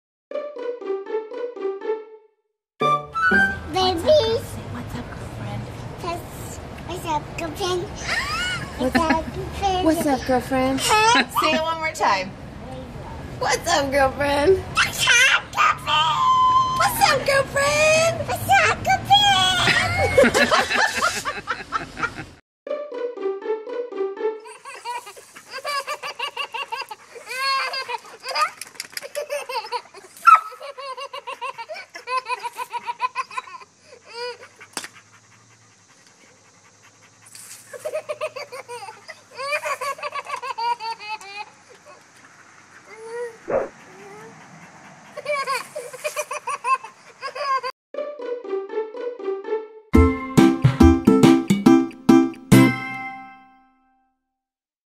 What's Up, Girlfriend? VS Baby Hoses Dog | Choose Your Feels
Which video pokes your heart between an adorable baby asking: "What's Up Girlfriend", and a sweet boy laughing as he sprays a dog with water?

Do you have a heartwarming video that you’d like to share?